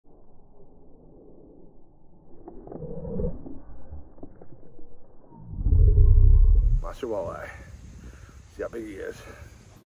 Super nice walleye smashes spinnerbait
This was a super cool walleye smash right by the boat. 🥰. River fishing Is the best